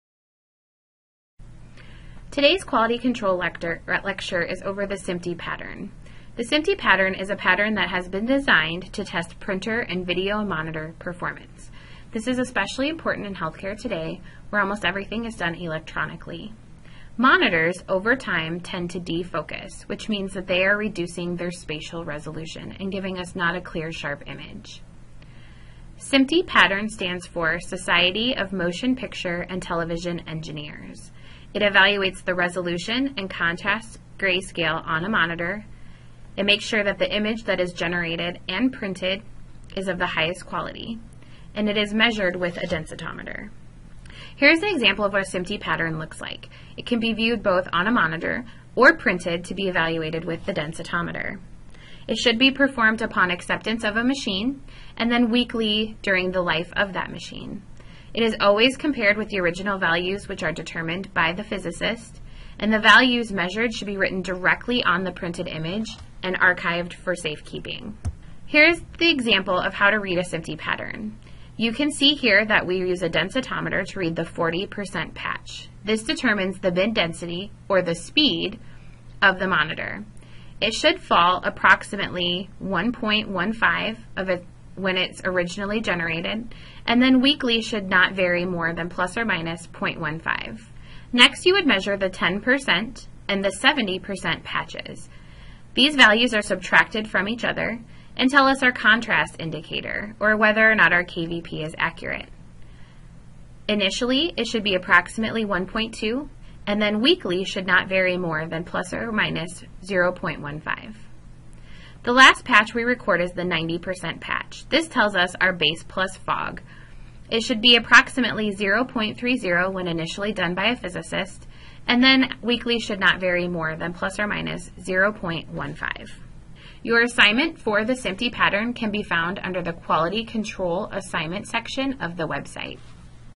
SMPTE Lecture
Instructional video on using and reading the SMPTE test pattern.